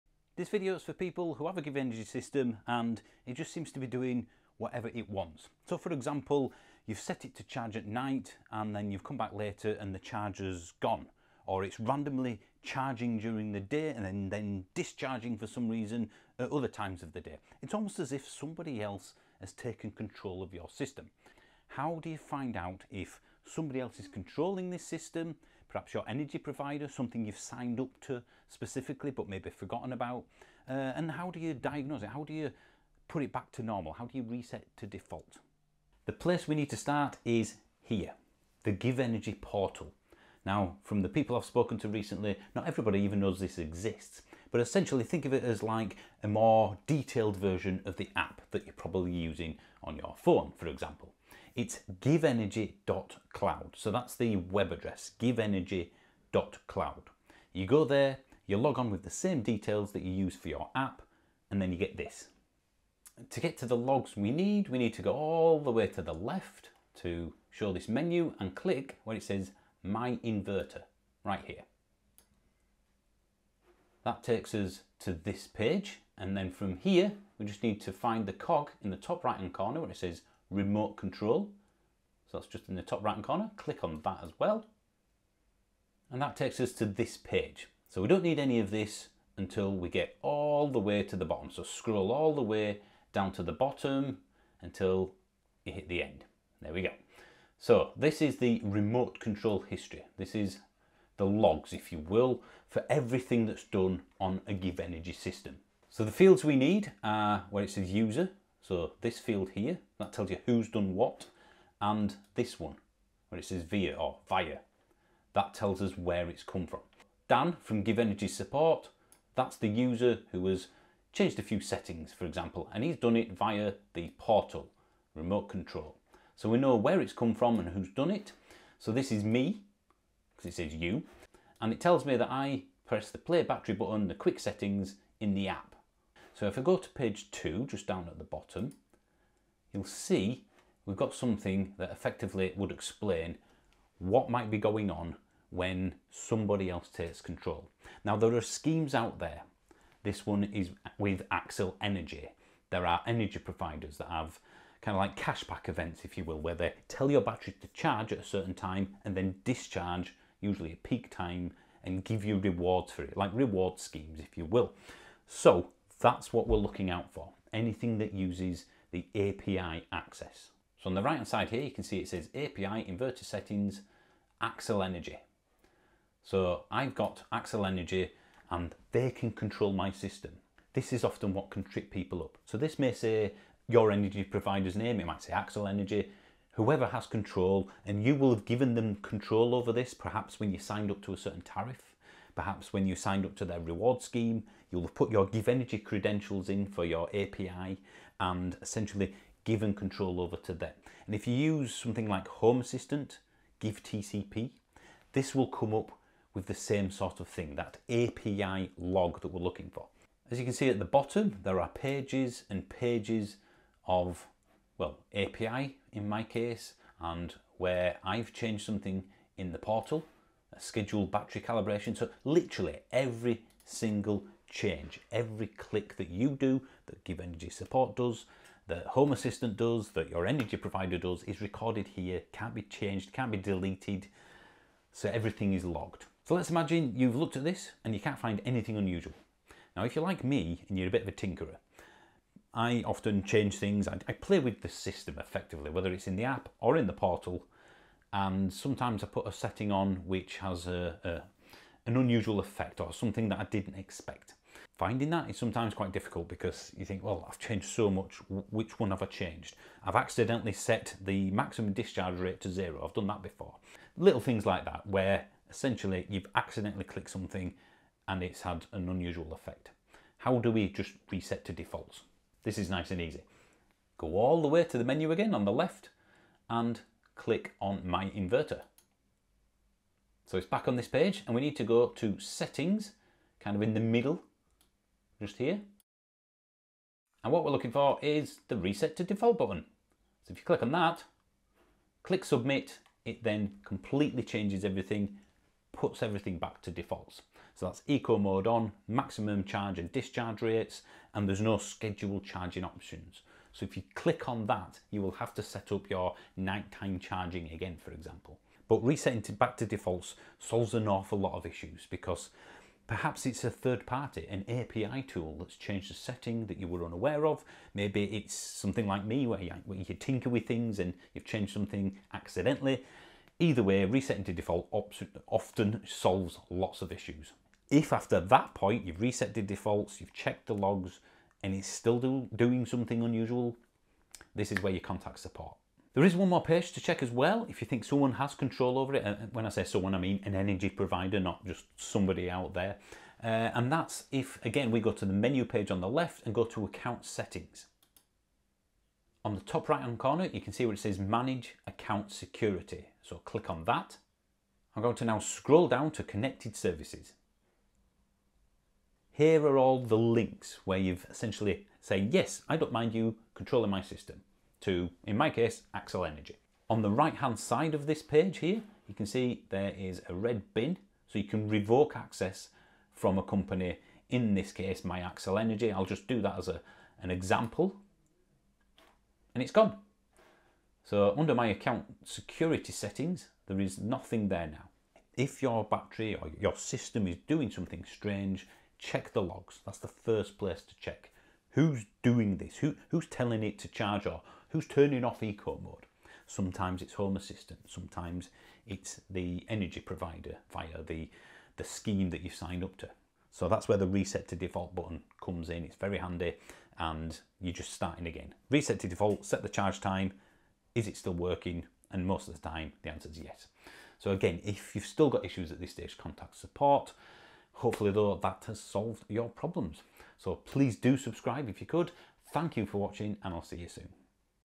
GivEnergy - Why Aren't My Charge Settings Staying? Finding Out What's Controlling Your System
If you've set times for charging and/or discharging but something keeps changing them, here's how to find out what's going on.